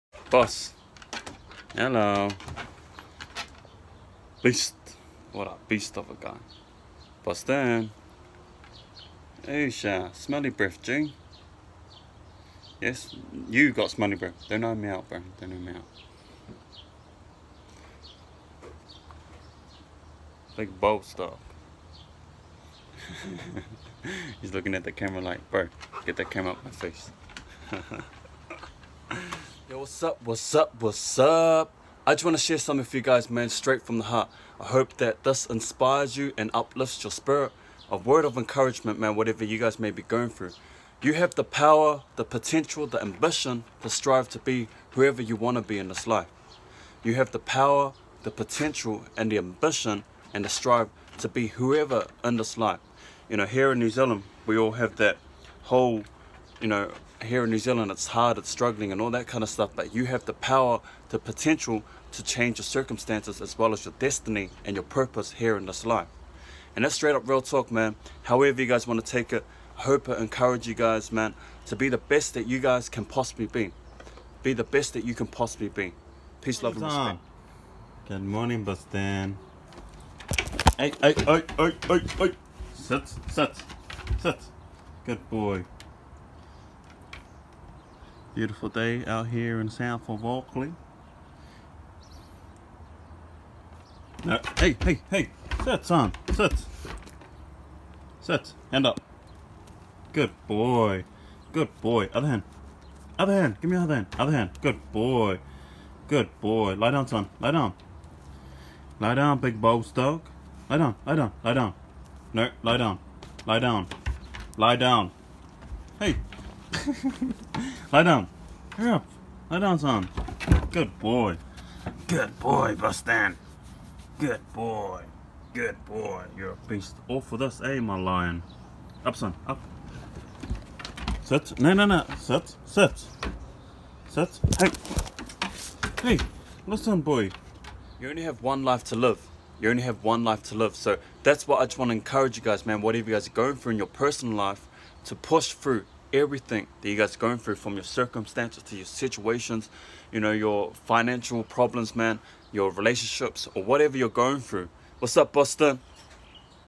“YOU HAVE THAT SAME POTENTIAL” 👻 INSPIAR_BABYY || 2018
Video 🎥 22/01/2018

“If you have a strong platform of INFLUENCE.. I encourage you.. LET IT SHINE (b)Right. ✨🌟💡🕯

🎥 Short little video Soon to be on YouTube

You have the Power the Potential the strive and ambition to change your destiny and purpose in this life, to change your situation and circumstance in this life and become the best that you can possibly be.
Hell its hard coming from New Zealand. Yes I know it’s hard here in NZ but there’s people out there still making a difference internationally as well as locally.. you can become that person to. I know all about struggle.

You only have one life to live, and one life to fulfill that purpose here in this world make a difference become the change, and be the influence you can possibly be. Leave a legacy of greatness.. you can become that. So I encourage you whatever you are going through in your own situation and life at the moment to push through and become that change that this world needs...
and become the best that you can possibly be.

There’s a lot of celebrities here in this world but not enough leaders. Like for myself I know I’m a natural born leader.. in this world we need natural born leaders to lead a nation lead a generation become that ripple effect I have had that calling spoken over my life when I was young and will lead where I can

These notes were written back in November 30th to December 1st

For me I want to become someone who leads a generation, or leads a nation making a Influence in peoples lives, I wouldn’t say become a role model because I’m far from it.. I’m real. I rather become a real model. As in share my struggles to show people you can push through! And Inspire But in order for that to happen I have to educate myself Visionary.

These are some mix videos that was meant to be on my snap chats, written a while ago to help and inspire people.. so I thought what if I combined all my videos into one and make it one whole video! I LIVE TO INSPIRE ❤️

🎥 NEW VIDEO SOON TO BE OM YOUTUBE WORDS OF ENCOURAGEMENT SUB TO MY YOUTUBE PAGE 😎 Please “You have the power, the strive, the ambition and the potential to change your destiny and purpose in this life..
even when people make excuses. “its so hard in NZ” that’s so true it is. But there’s still people out there making a difference in Little ol New Zealand. I encourage you become that person who makes a difference, not just in yourself and well being but for others to. You would know all about struggle if you come from the Land of the long white cloud. ⛅️ ☁️